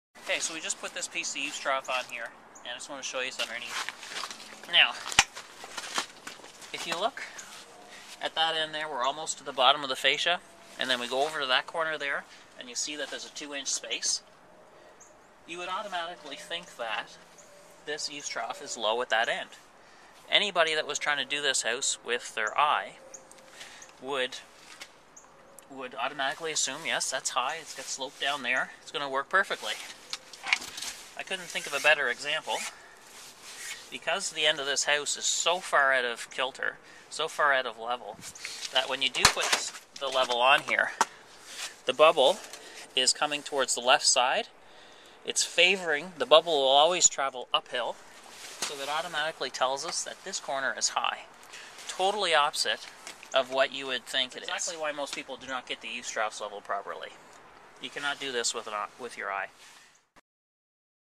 Using A Level Is A Must
Installers that install eavestroughs by eye without a level can easily make a mess of your eavestroughs leaving you with new gutters that hold water.
Not all houses sit perfectly level and there is no way to tell unless the installers use a level.